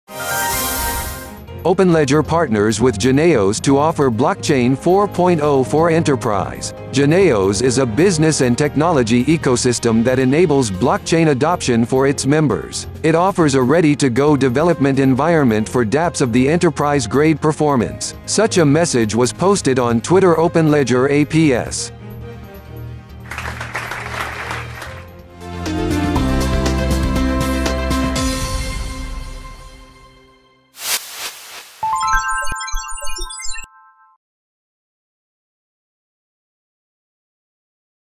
genEOS. Blockchain 4.0 – Crowdsale is launched
OpenLedger Partners with genEOS to Offer Blockchain 4.0 for Enterprise. genEOS is a business and technology ecosystem that enables blockchain adoption for its members. It offers a ready-to-go development environment for Dapps of the enterprise-grade performance. Such a message was posted on Twitter OpenLedger ApS.